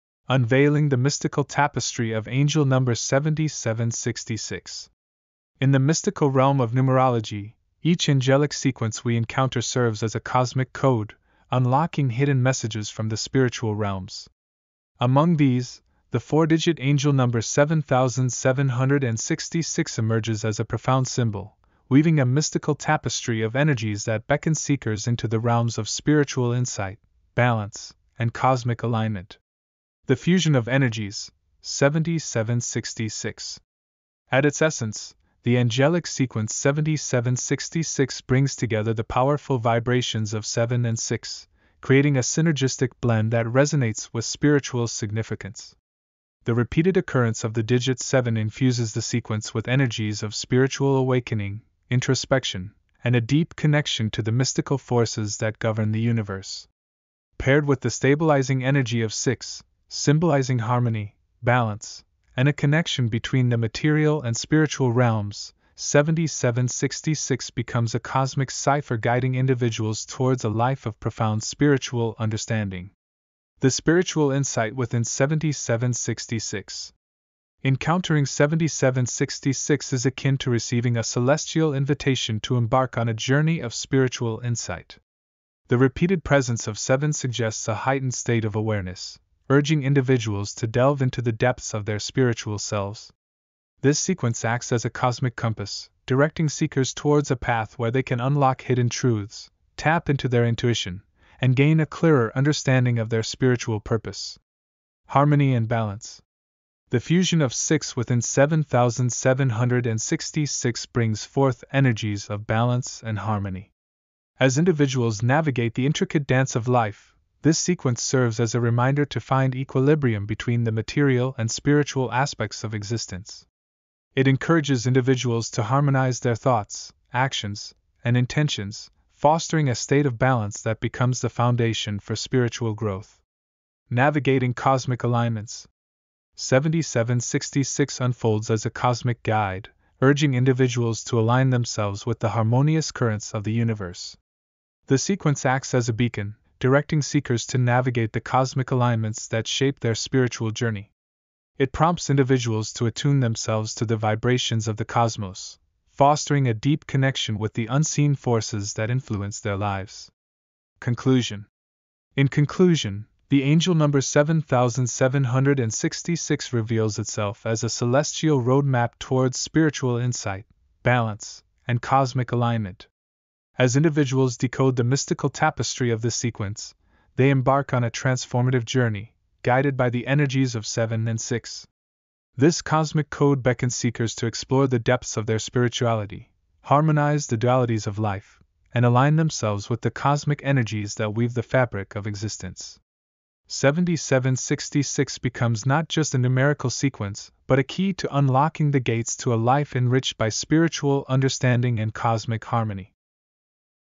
Angel Number 7766 – Meaning and Symbolism 💕
Unveiling the Mystical Tapestry of Angel Number 7766
In the mystical realm of numerology, each angelic sequence we encounter serves as a cosmic code, unlocking hidden messages from the spiritual realms. Among these, the four-digit angel number 7766 emerges as a profound symbol, weaving a mystical tapestry of energies that beckon seekers into the realms of spiritual insight, balance, and cosmic alignment.

The Fusion of Energies: 7766
At its essence, the angelic sequence 7766 brings together the powerful vibrations of 7 and 6, creating a synergistic blend that resonates with spiritual significance. The repeated occurrence of the digit 7 infuses the sequence with energies of spiritual awakening, introspection, and a deep connection to the mystical forces that govern the universe. Paired with the stabilizing energy of 6, symbolizing harmony, balance, and a connection between the material and spiritual realms, 7766 becomes a cosmic cipher guiding individuals towards a life of profound spiritual understanding.

The Spiritual Insight Within 7766
Encountering 7766 is akin to receiving a celestial invitation to embark on a journey of spiritual insight. The repeated presence of 7 suggests a heightened state of awareness, urging individuals to delve into the depths of their spiritual selves. This sequence acts as a cosmic compass, directing seekers towards a path where they can unlock hidden truths, tap into their intuition, and gain a clearer understanding of their spiritual purpose.

Harmony and Balance
The fusion of 6 within 7766 brings forth energies of balance and harmony. As individuals navigate the intricate dance of life, this sequence serves as a reminder to find equilibrium between the material and spiritual aspects of existence. It encourages individuals to harmonize their thoughts, actions, and intentions, fostering a state of balance that becomes the foundation for spiritual growth.

Navigating Cosmic Alignments
7766 unfolds as a cosmic guide, urging individuals to align themselves with the harmonious currents of the universe. The sequence acts as a beacon, directing seekers to navigate the cosmic alignments that shape their spiritual journey. It prompts individuals to attune themselves to the vibrations of the cosmos, fostering a deep connection with the unseen forces that influence their lives.

Conclusion
In conclusion, the angel number 7766 reveals itself as a celestial roadmap towards spiritual insight, balance, and cosmic alignment. As individuals decode the mystical tapestry of this sequence, they embark on a transformative journey, guided by the energies of 7 and 6. This cosmic code beckons seekers to explore the depths of their spirituality, harmonize the dualities of life, and align themselves with the cosmic energies that weave the fabric of existence. 7766 becomes not just a numerical sequence but a key to unlocking the gates to a life enriched by spiritual understanding and cosmic harmony.